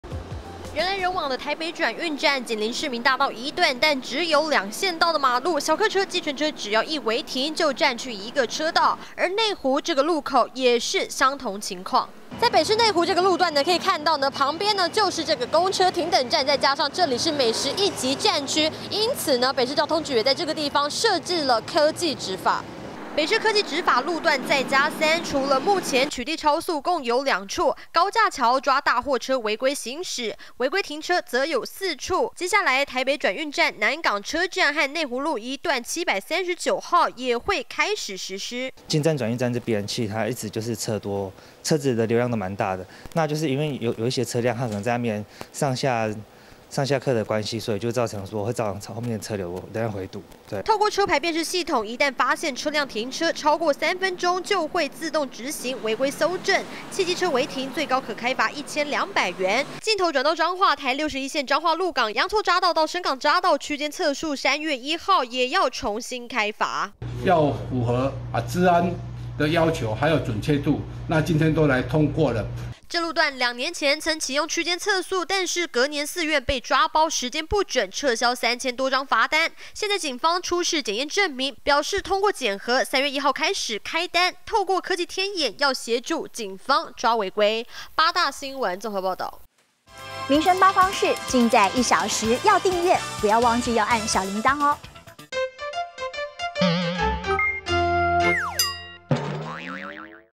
科技執法抓違停 北市3/1再增3處 八大民生新聞 2022022801
運用科技執法取締違規停車，原本只有4處路口有設置，但現在包括台北轉運站、南港車站以及內湖路1段739號前，都將在3月1號正式啟用，而在彰化鹿港，有隻號稱是吸金王的區間測速，也從1號開始重新取締，將近9公里距離，要是經過時間，不到5分21秒，就超速了。